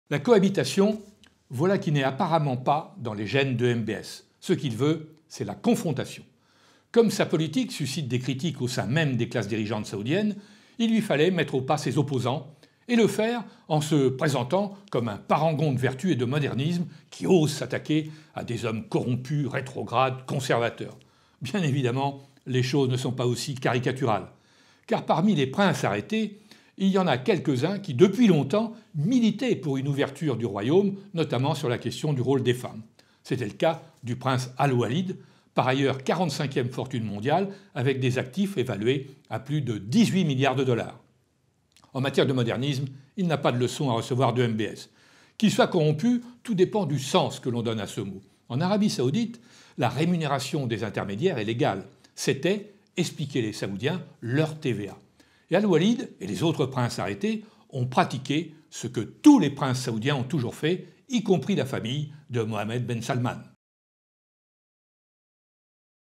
Les jeux dangereux de MBS - Polony TV
Mohammed Ben Salmane purge la classe dirigeante saoudienne et force à la démission le premier ministre du Liban. Il renforce son pouvoir, complaît aux Américains et poursuit son offensive contre l'Iran.

Polony TV le média libre de la France souveraine